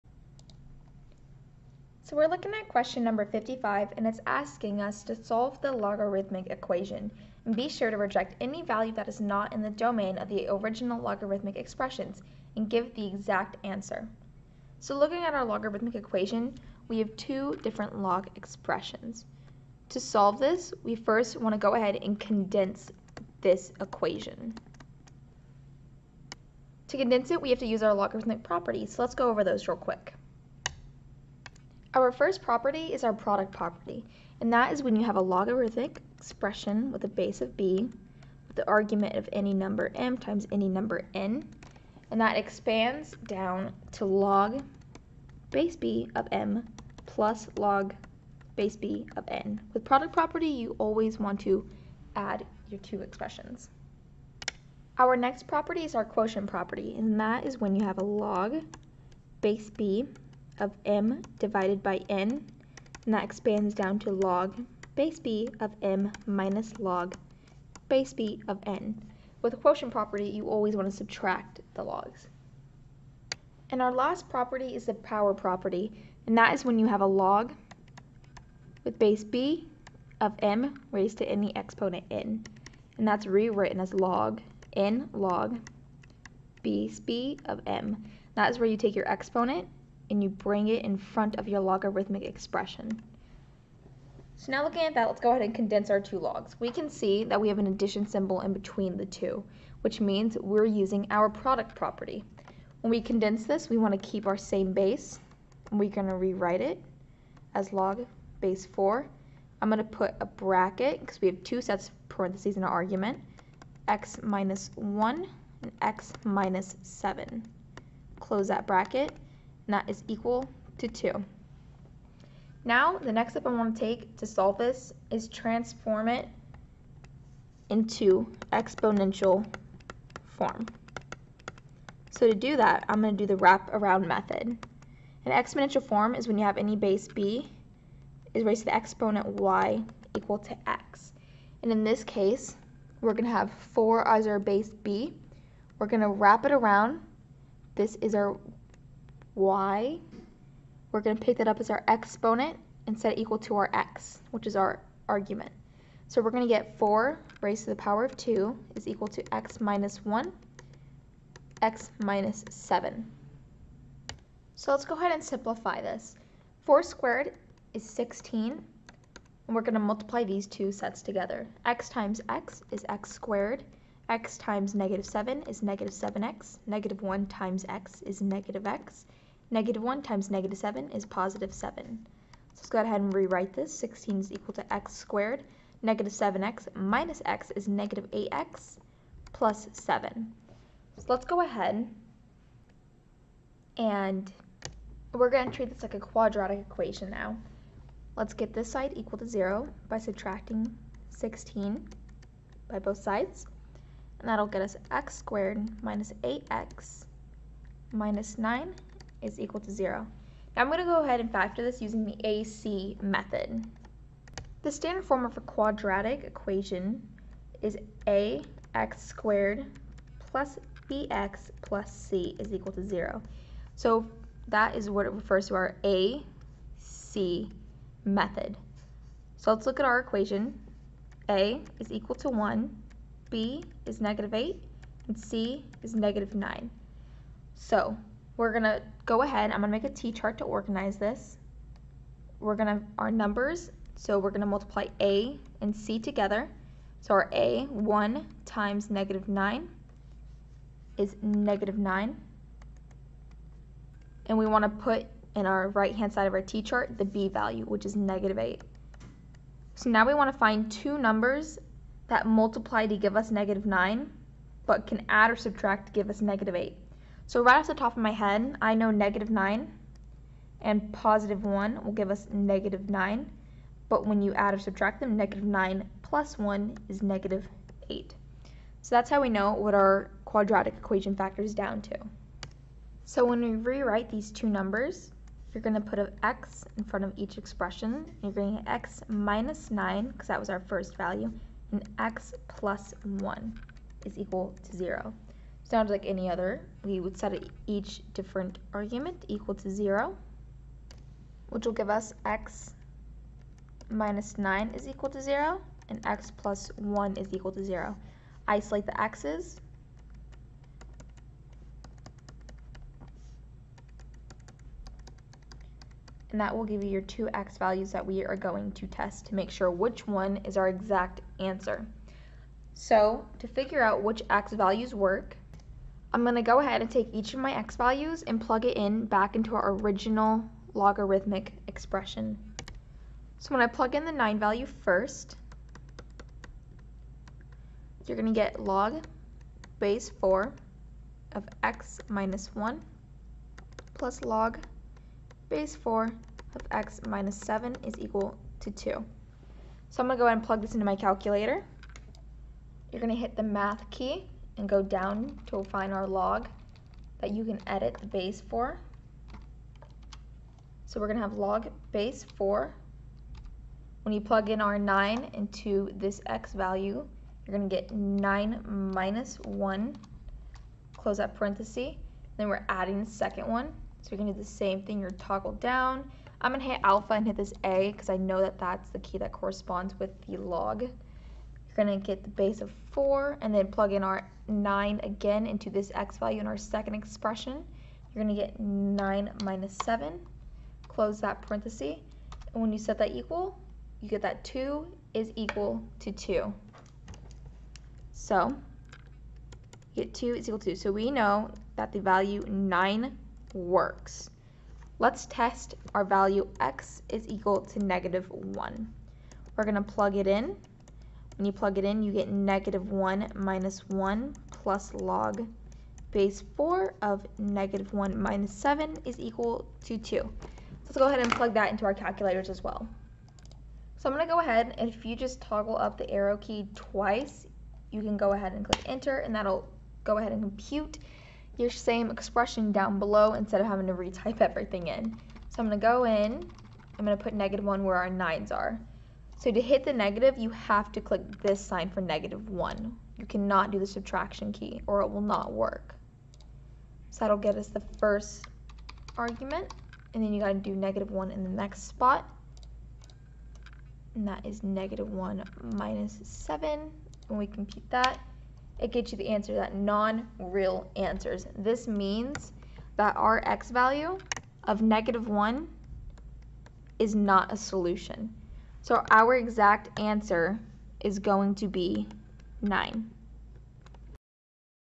55) Solve the logarithmic equation...
55) Solve the logarithmic equation. Be sure to reject any value that is not in the domain of the original logarithmic expressions. Give the exact answer.

Created by: Dallas Myers